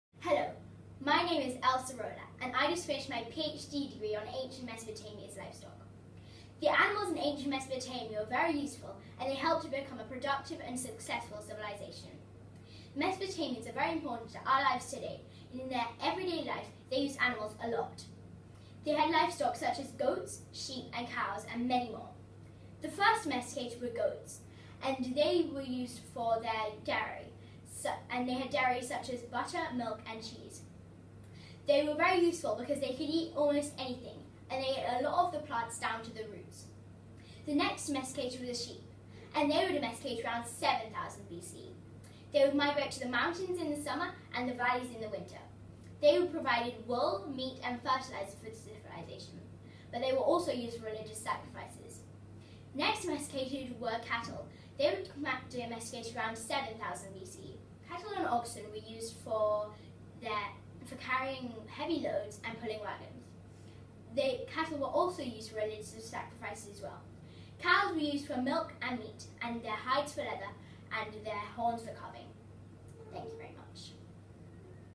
Voices of the River Valleys - Mesopotamia Livestock by Elsa
I chose this aspect of Mesopotamia because it was very interesting to see how the animals helped the villagers in their everyday life. It was also an interesting to see what type of animals were used for their meat, and what types of animals were used for actual jobs. All the animals seemed to have a very clear role in the civilisation and the roles were very important as well. Without the animals the civilisation it wouldn’t be as easy and they would have to work harder. Also what interested me was how much information I could find online because when I chose this aspect I was not sure if I could find enough information.